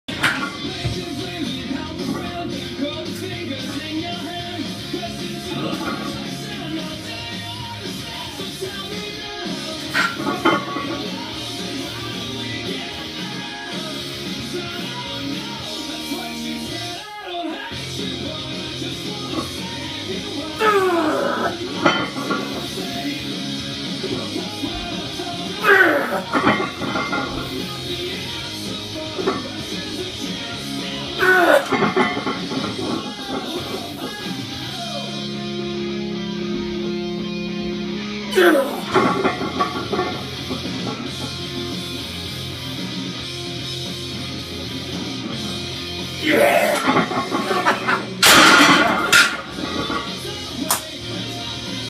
170kg x 5 Squat
Did 2x5 at 175kg then 3x5 at this 170kg. Best volume work I've done on squats